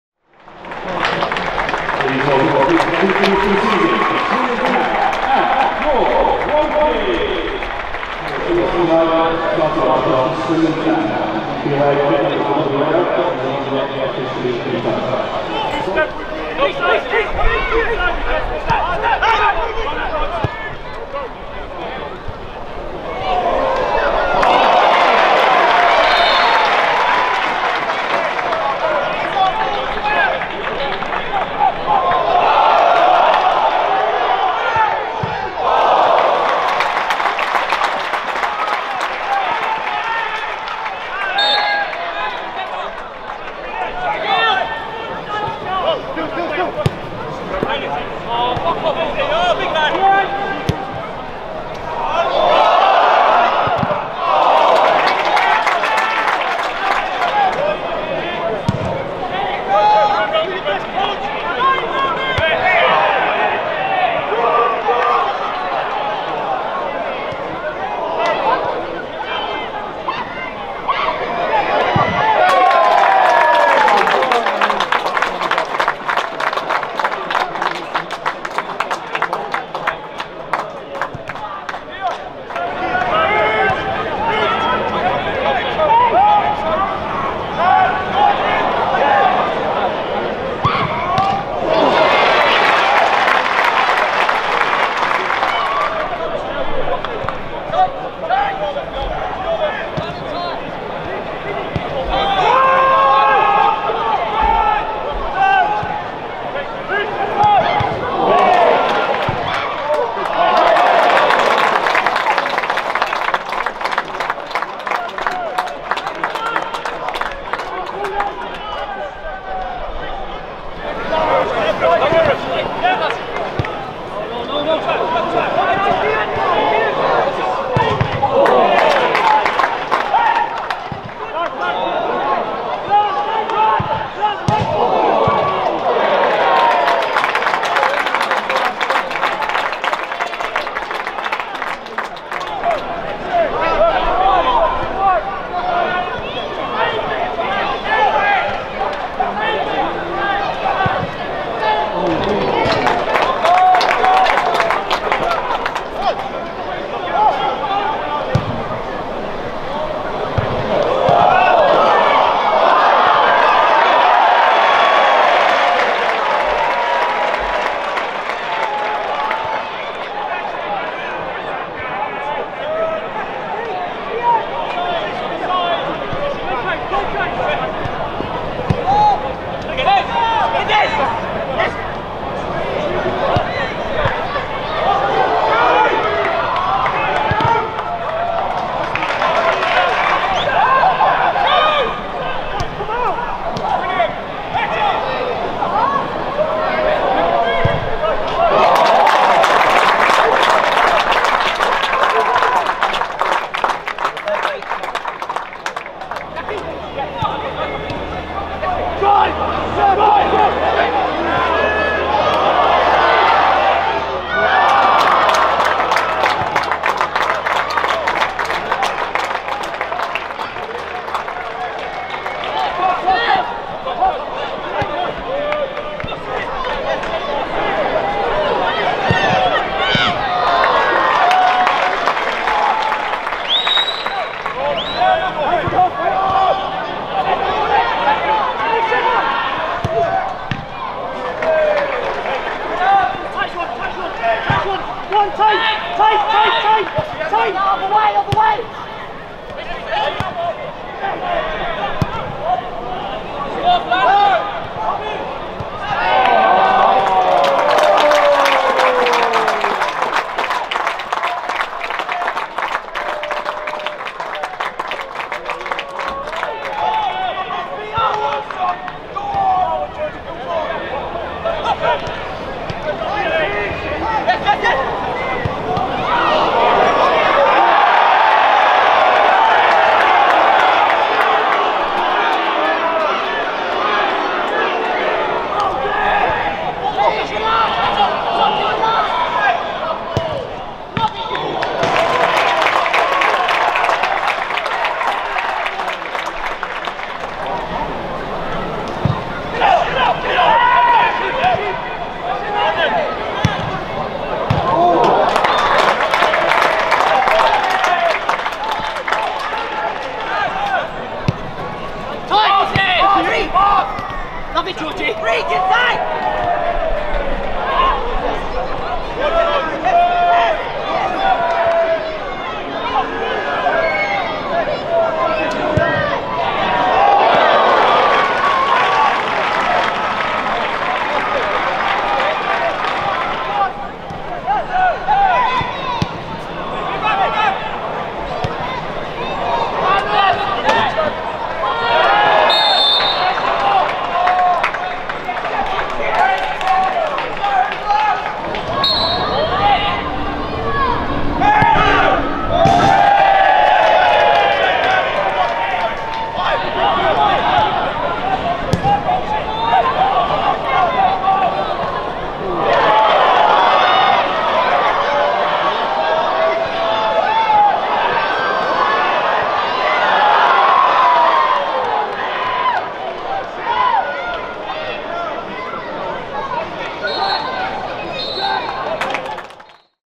Woking 0 - 4 Swindon Town | Match Highlights
The key moments as Woking fell to a 4-0 defeat against a strong Swindon Town side in pre-season.